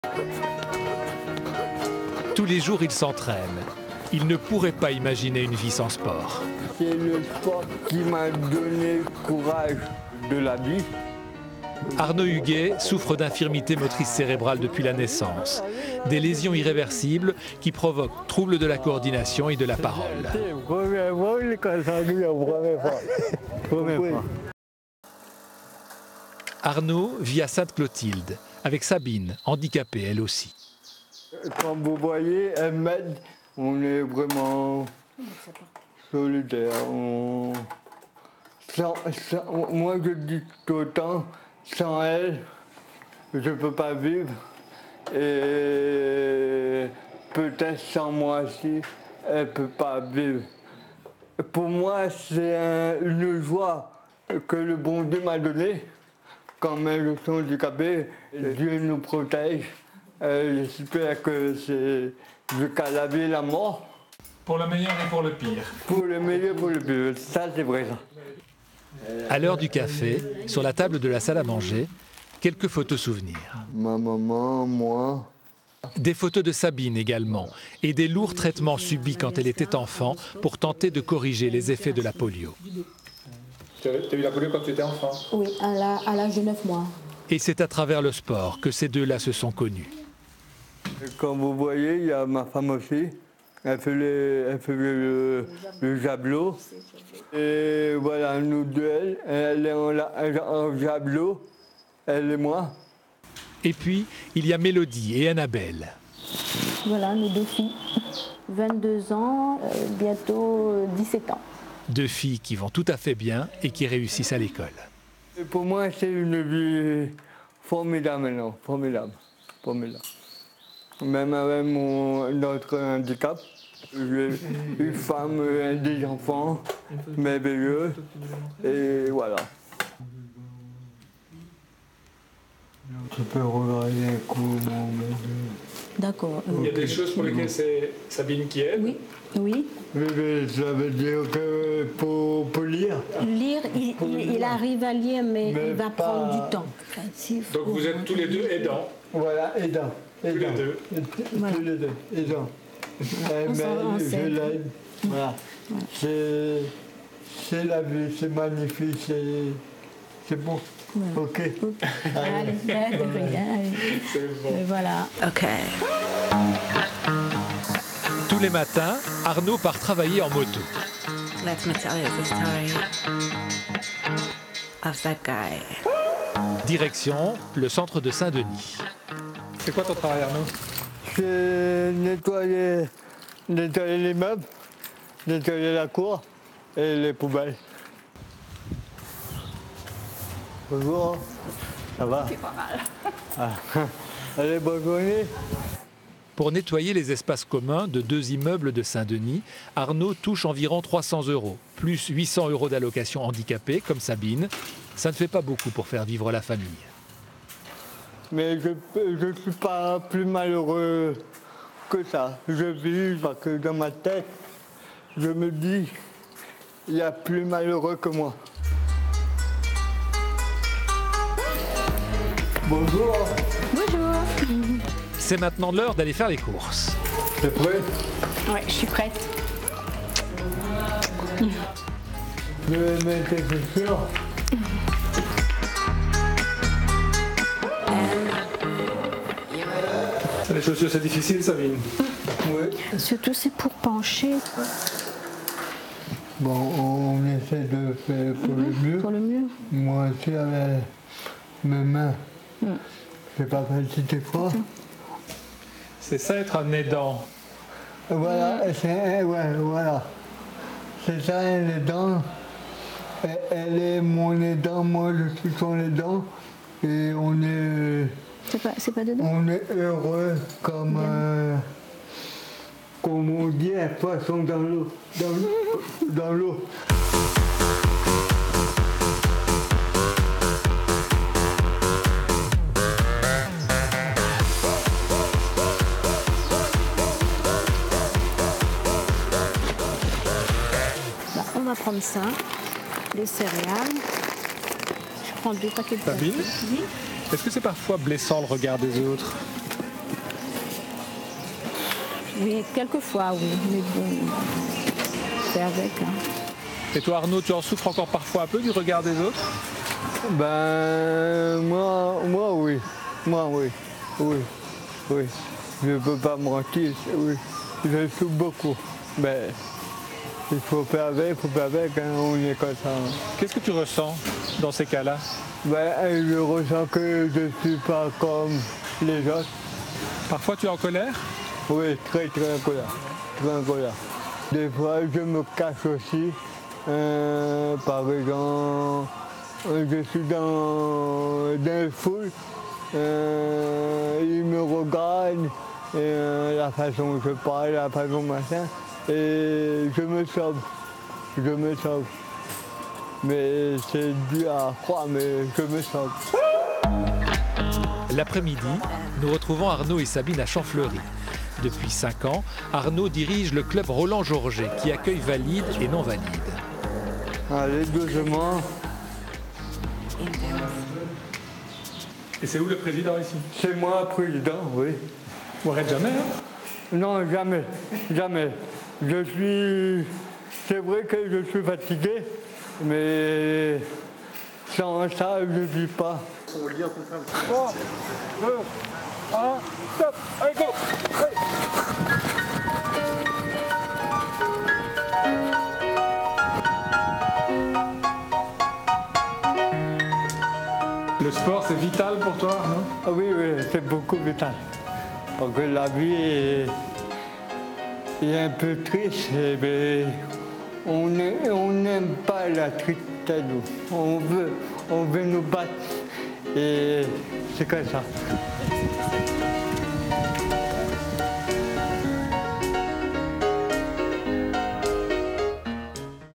UN JOUR AVEC... Arnaud “ aidant et handicapé ”
Découvrez la force et la joie de vivre de Arnaud avec ce reportage de Jean-Marc Collienne, Laurent Figon et Bruno Rosalie.